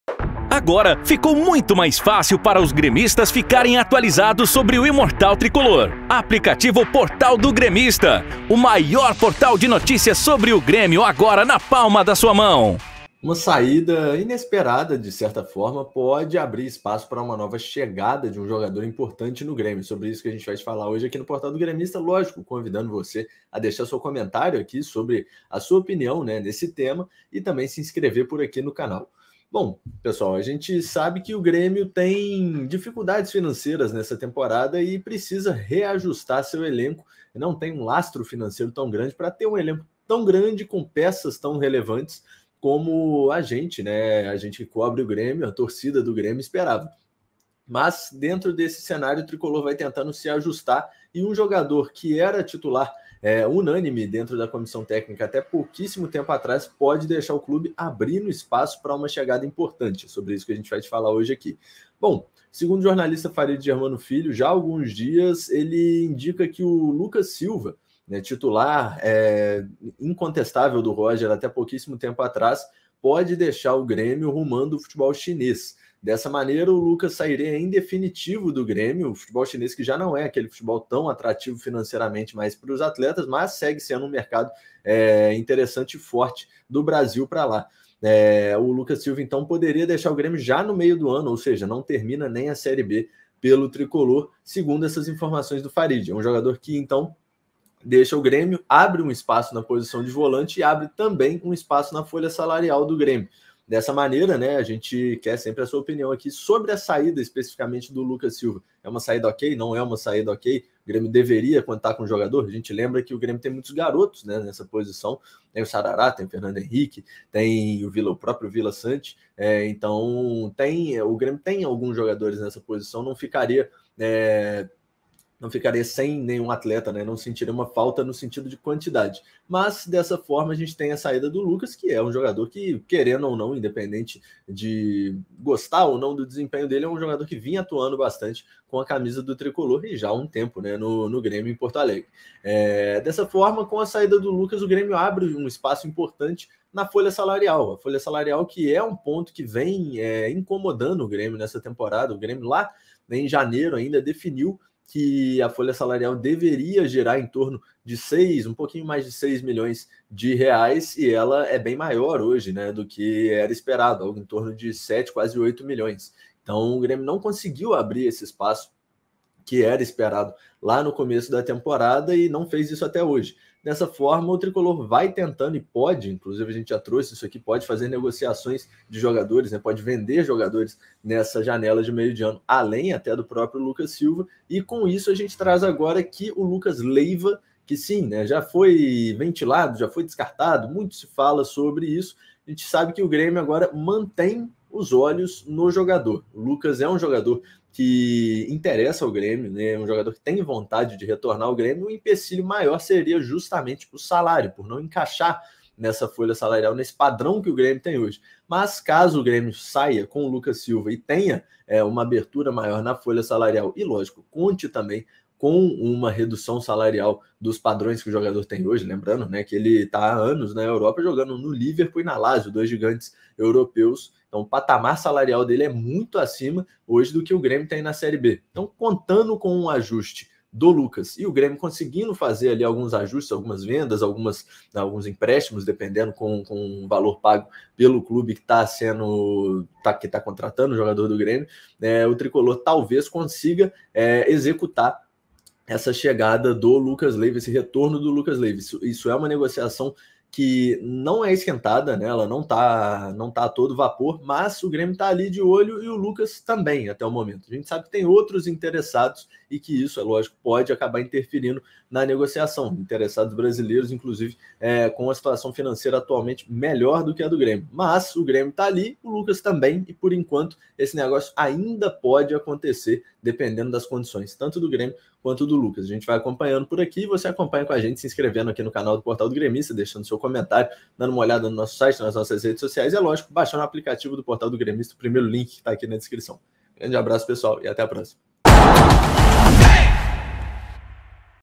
Grêmio deve perder jogador e abrir espaço para nova contratação importante!
O Tricolor pode perder um jogador relevante na janela de meio de ano. Apesar disso, a saída pode abrir espaço para uma chegada relevante.